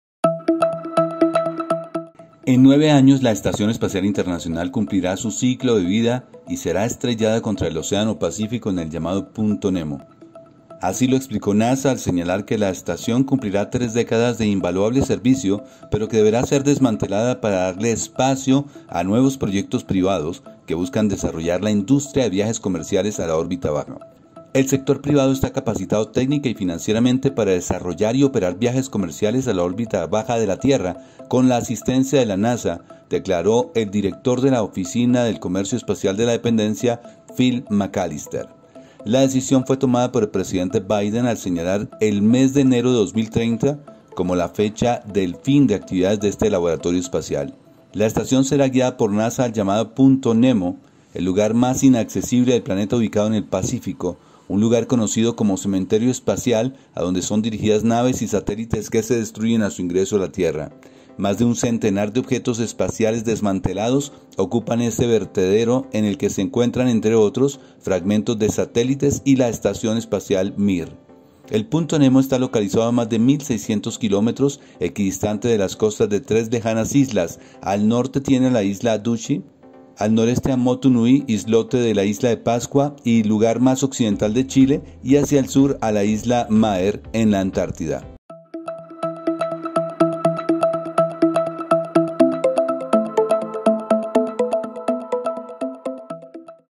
ATENCIÒN: NASA fijó año para estrellar la Estación Espacial Internacional tras décadas de servicio
En 9 años la Estación Espacial Internacional cumplirá su ciclo de vida y será estrellada contra el océano pacífico en el llamado punto NEMO
Así lo explicó NASA al señalar que la estación cumplirá tres décadas de invaluable servicio pero que deberá ser desmantelada para darle espacio a nuevos proyectos privados que buscan desarrollar la industria de viajes comerciales a la órbita baja.
«El sector privado está capacitado técnica y financieramente para desarrollar y operar viajes comerciales a la órbita baja de la Tierra, con la asistencia de la NASA», declaró en tanto el director de las oficinas de comercio espacial de la dependencia, Phil McAlister.
La decisión fue tomada por el presidente Biden al señalar el mes de enero de 2030 como la fecha del fin de actividades de este laboratorio espacial.
La estación será guiada por NASA al llamado punto NEMO, el lugar más inaccesible del planeta ubicado en el Pacífico. Un lugar conocido como Cementerio Espacial a donde son dirigidas naves y satélites que se destruyen a su ingreso a la Tierra.
Más de un centenar de objetos espaciales desmantelados ocupan ese vertedero en el que se encuentran, entre otros, fragmentos de satélites y de la estación espacial Mir.
El Punto Nemo está localizado a más de 1.600 kilómetros equidistante de las costas de tres lejanas islas. Al norte tiene a la Isla Ducie (una de las islas Pitcairn), al noreste a Motu Nui (islote de la Isla de Pascua y lugar más occidental de Chile) y hacia el sur a la Isla Maher (Antártida).